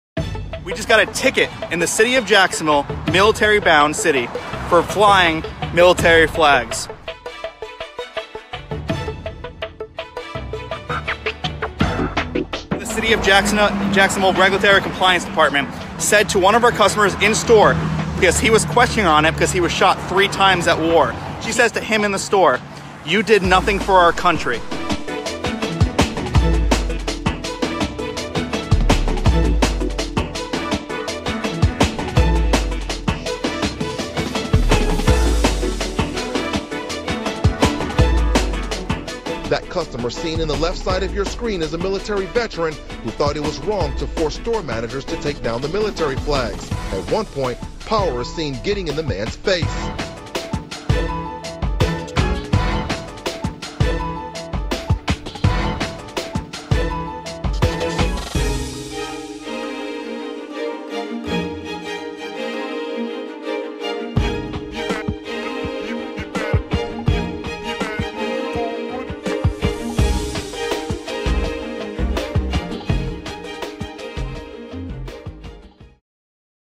Snopes.com: Flag Controversy at Jaguar Power Sports in Florida
City of Jacksonville employee Melinda Power was targeted by Jaguar Power Sports after citing the store for violating local signage codes.